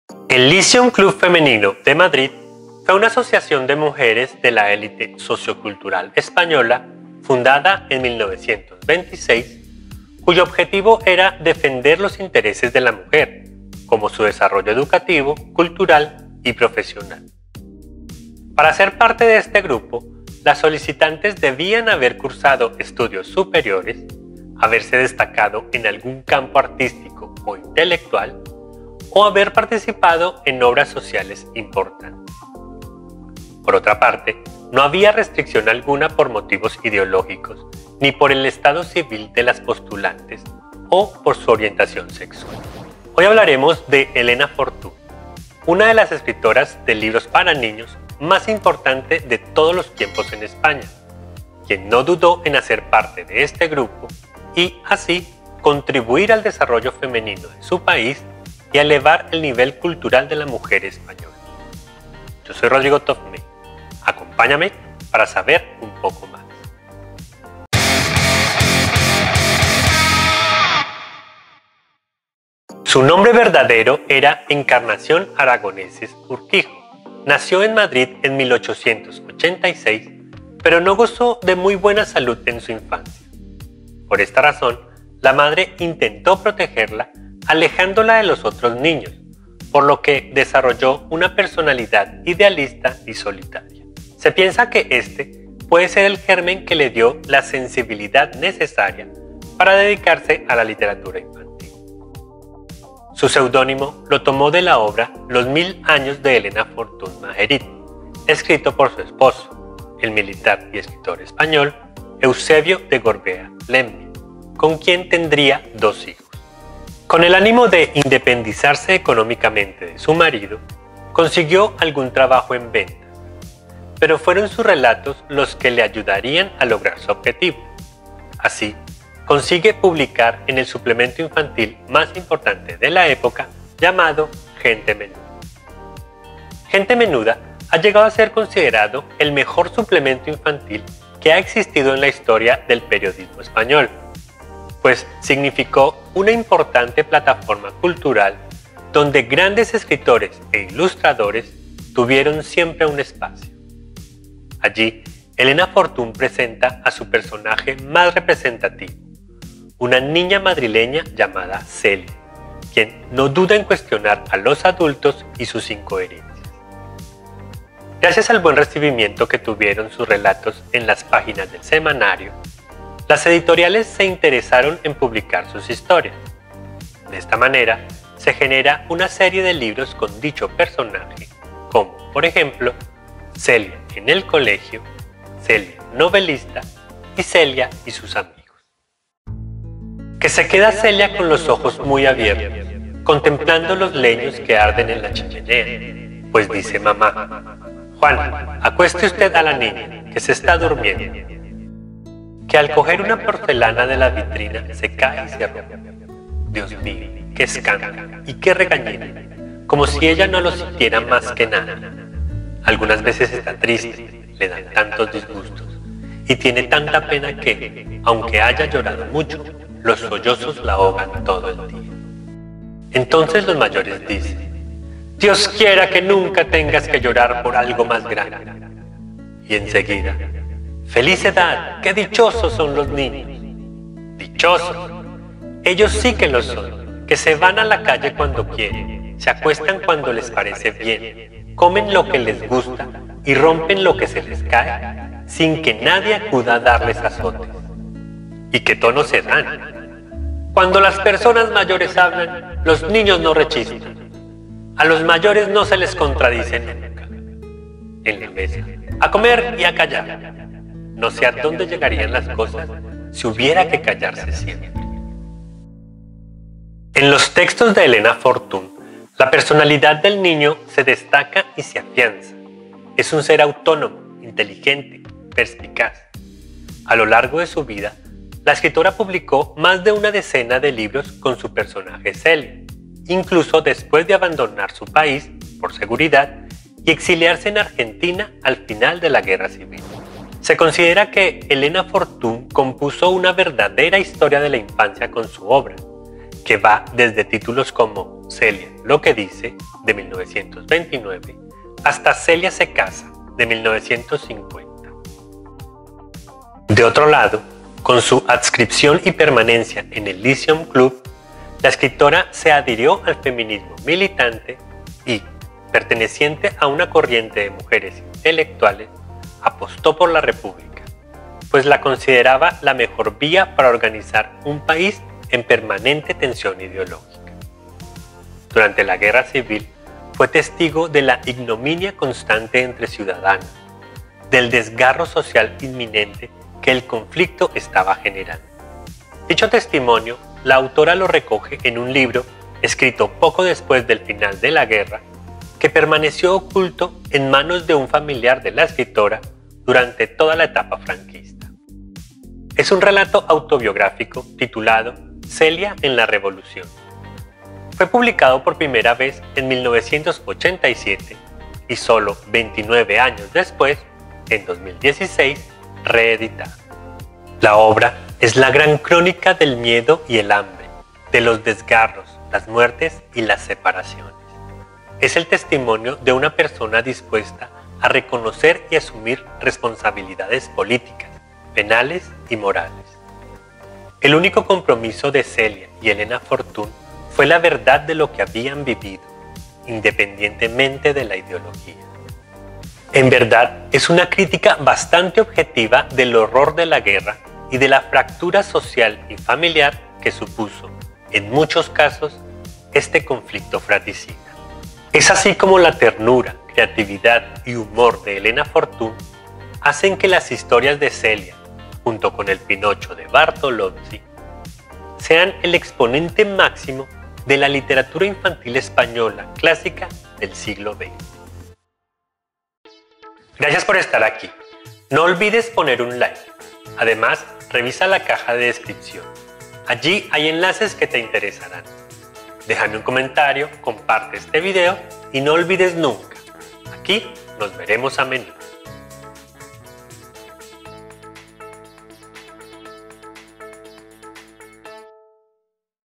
Celia de Elena Fortún, el exponente máximo de la literatura infantil clásica del siglo XX en España
La serie de libros de Celia de Elena Fortún es considerada una joya de la literatura infantil. Con su personaje Celia, Elena Fortún logra crear toda una historia de la infancia donde todo profesional de las áreas concernientes a la niñez podría encontrar referencias útiles.

Enlaces de interés:

Vídeo de Manos Orfanos en Pexels
Vídeo de Monstera en Pexels
Vídeo de cottonbro en Pexels
Vídeo de Rob en Pexels
Vídeo de Matthias Groeneveld en Pexels
Vídeo de Taryn Elliott en Pexels